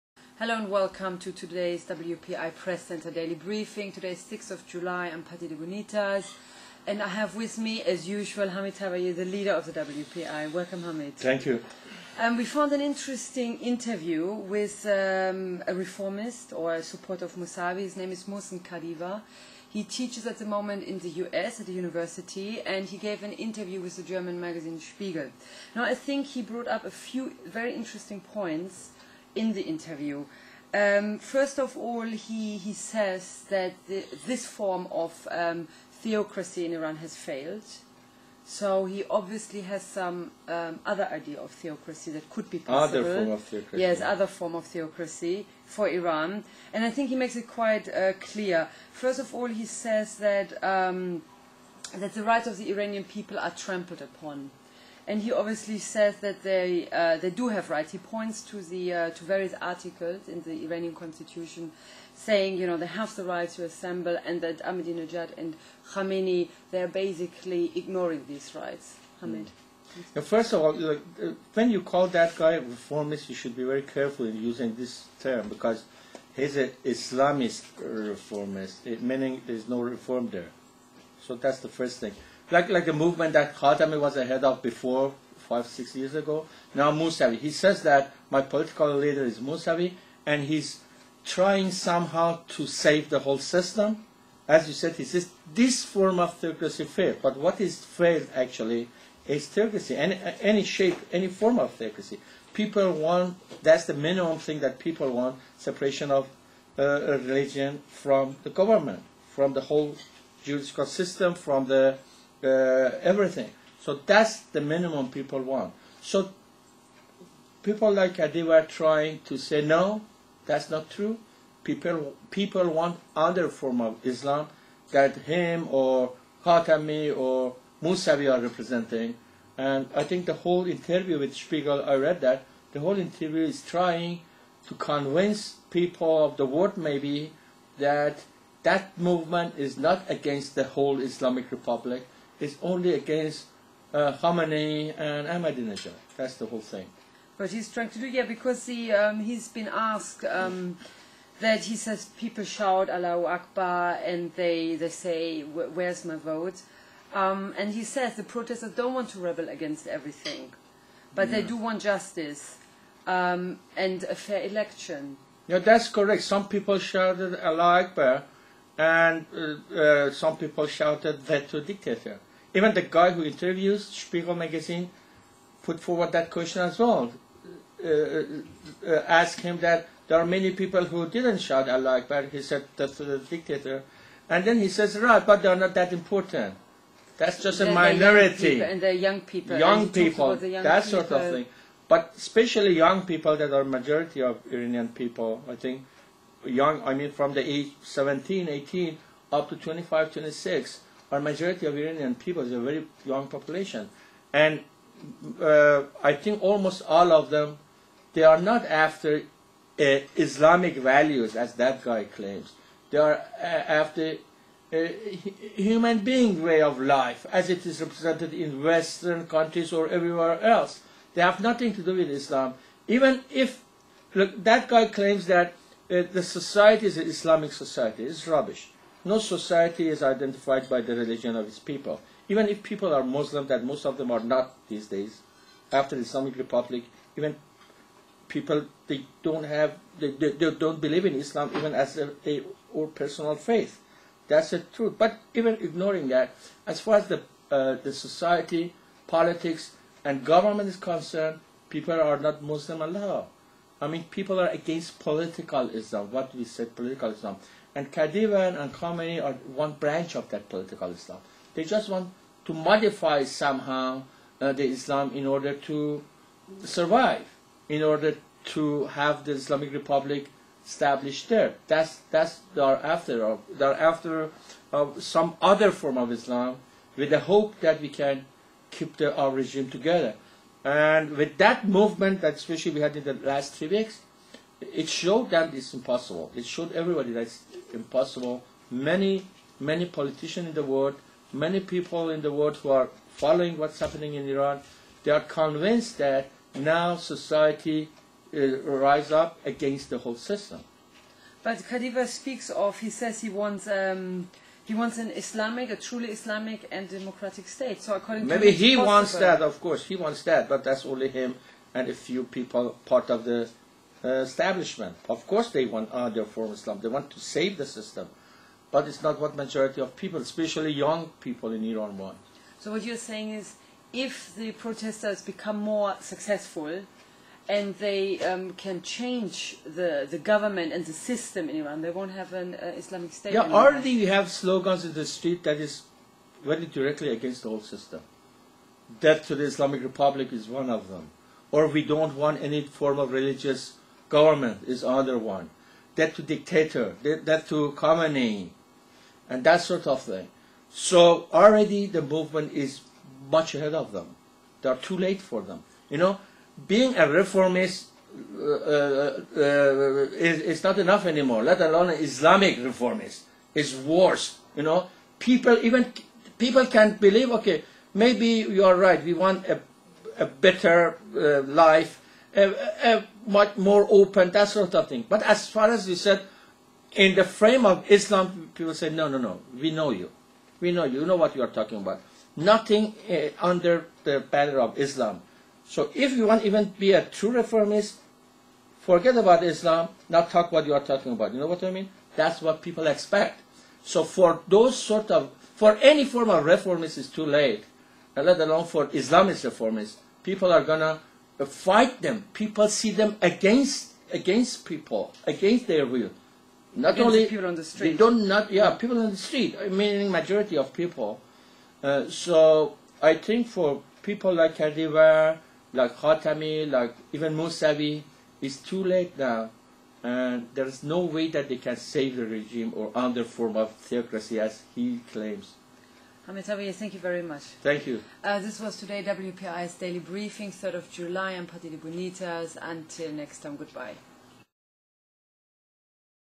'Islamic reform' in Iran?
In today's programme Hamid Taqvaee responds to statements made by Mohsen Kadivar, a supporter of  Mousavi, on the current political situation in Iran. Is an  'Islamic reform' possible in Iran? WPI press centre daily dispatch, 3 July 2009.